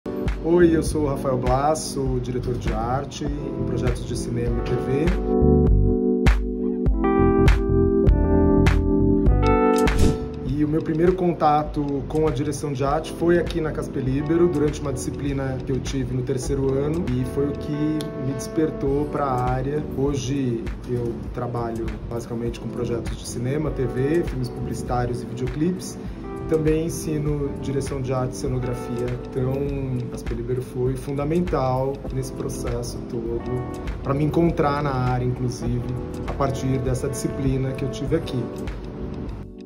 Rafael Blas conta como escolheu trabalhar na área de Direção e Arte
Rafael Blas, formado em Rádio e TV na Cásper, está há mais de 19 anos no mercado audiovisual. O casperiano foi assistente de grandes nomes da direção de arte cinematográfica, e fala sobre a importância da Faculdade na sua trajetória profissional. Confira!